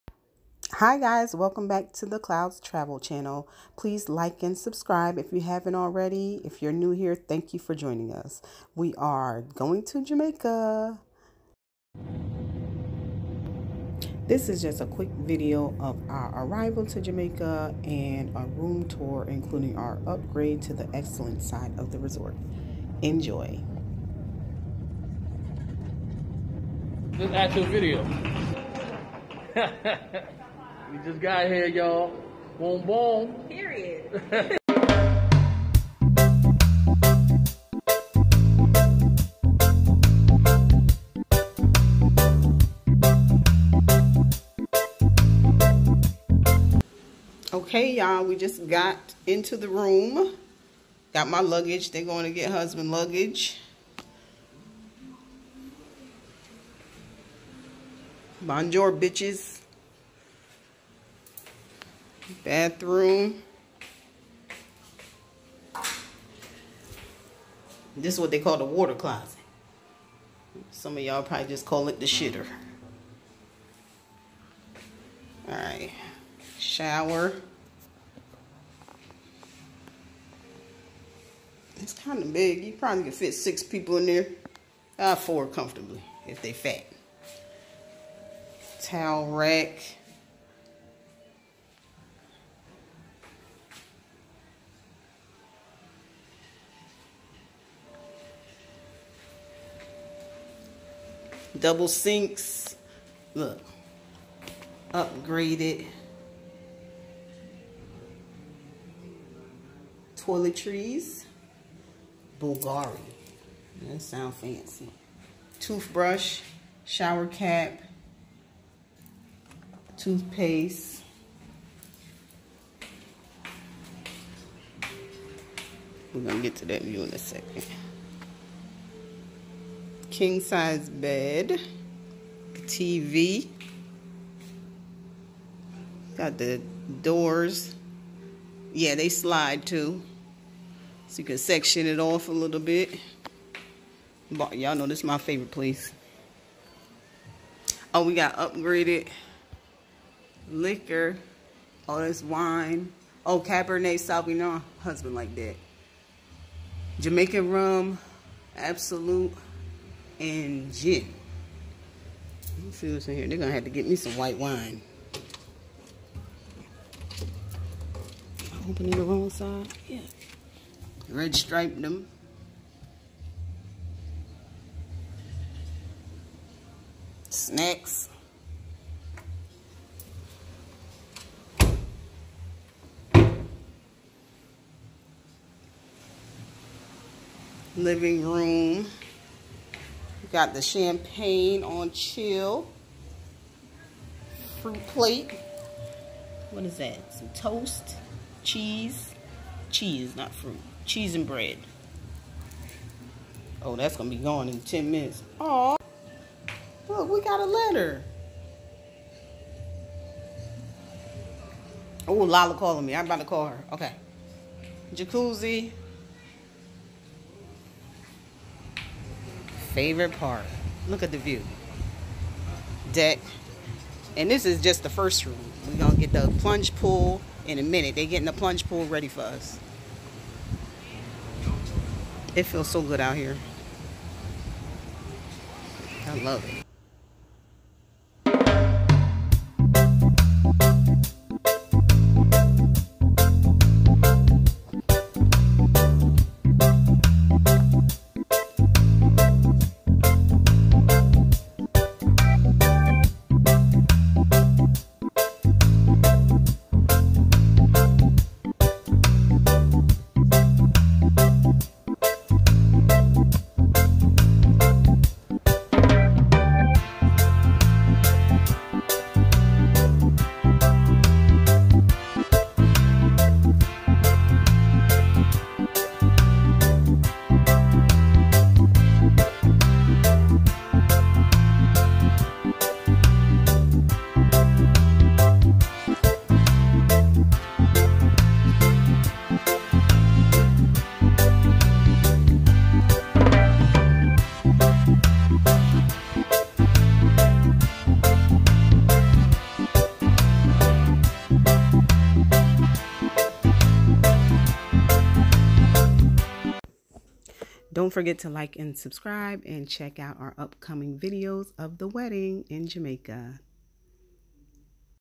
Excellence Oyster Bay Room Tour // Upgrade to Excellence Club Ocean Front Plunge Pool
@travelingclouds Finally back in Jamaica for our wedding!  Here's a short room tour of our upgrade to the Excellence Club Plunge Pool